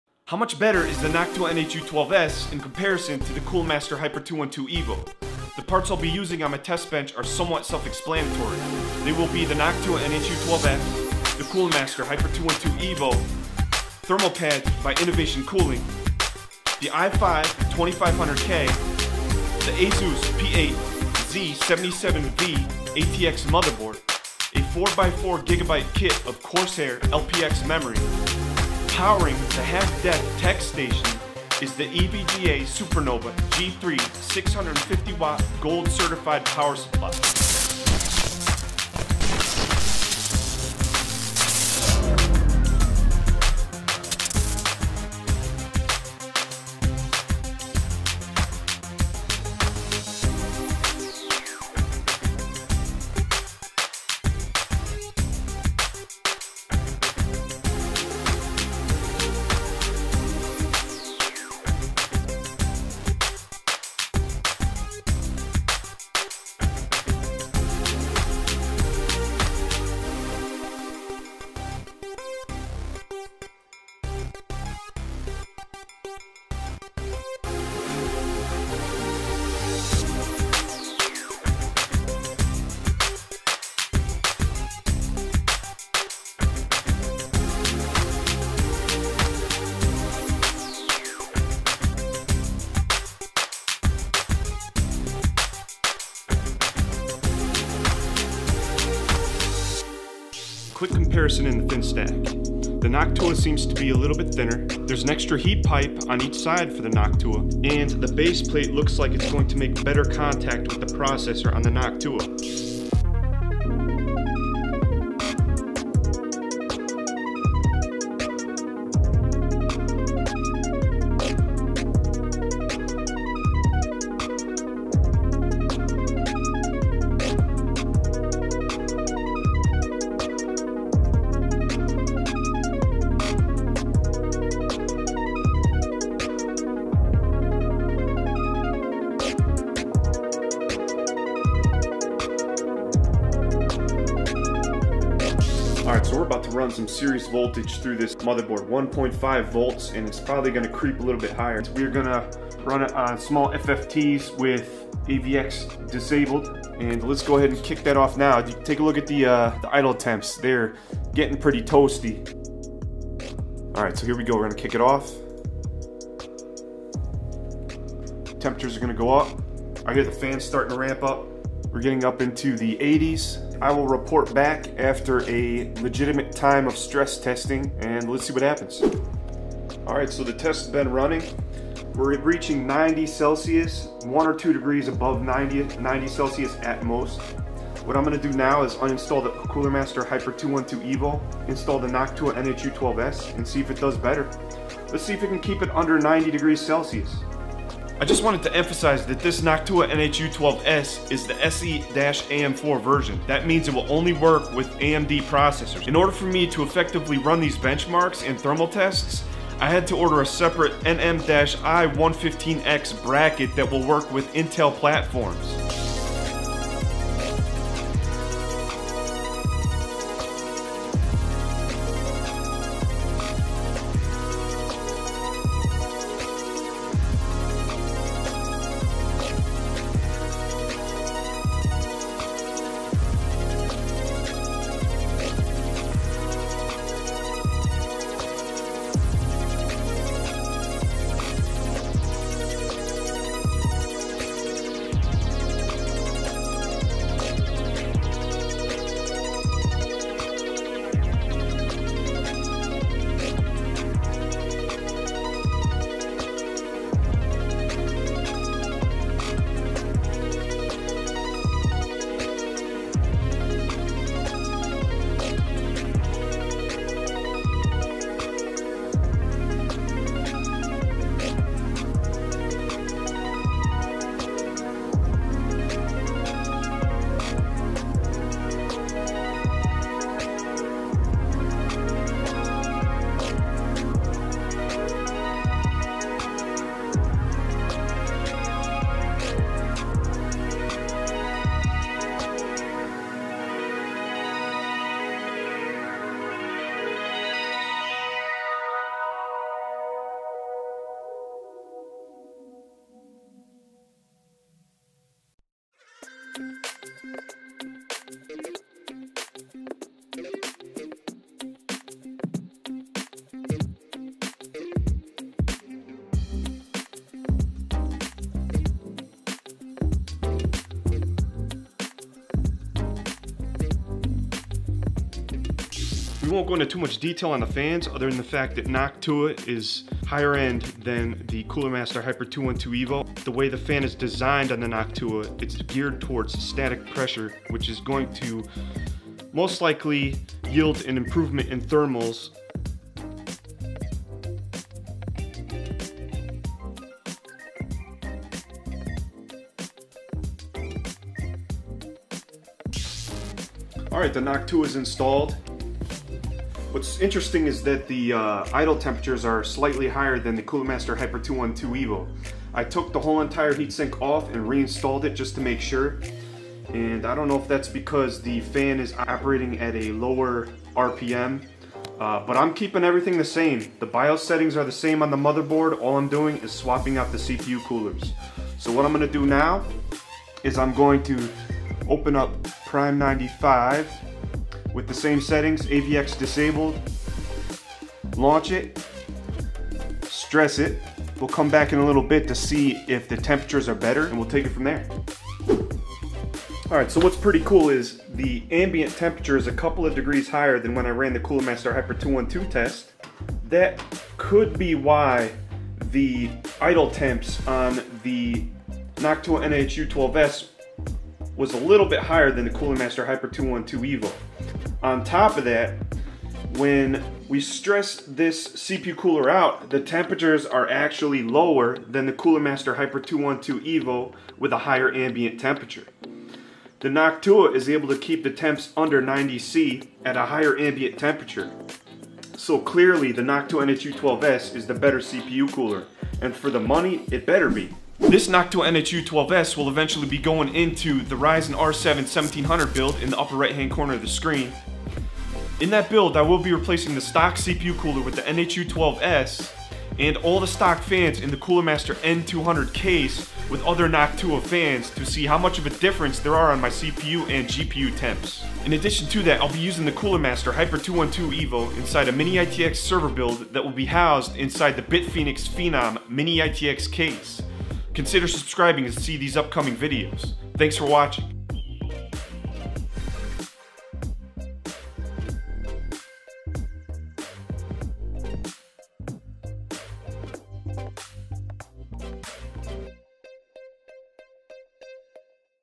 Cooler Master Hyper 212 Evo VS Noctua NHU12S Overclocked to 4.9 ghz at 1.5v - Who will win?
How much better is the Noctua NHU12S in comparison to the $20 Cooler Master Hyper 212 Evo?  Join me in this video to find out the results and discover if the $60 premium price tag of Noctua is worth it.
Cooler Master Hyper 212 Evo
Noctual NH-U12S
EVGA SuperNova G3 650w Gold Certified Power Supply

Processor: Intel Core i5 2500k
Motherboard: Asus P8Z77-V
Memory: Corsair LPX DDR3 1600 Mhz

Bitfenix Phenom Mini ITX Case